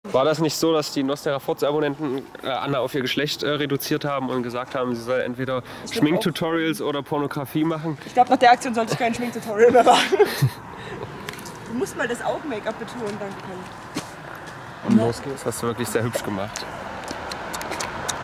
Da habt ihr's, ihr Molche.
Anders bekommt sie ja keine Aufmerksamkeit von euch :-/
Letzter Teaser vor'm großen GTA 5 Real Life Release am Samstag.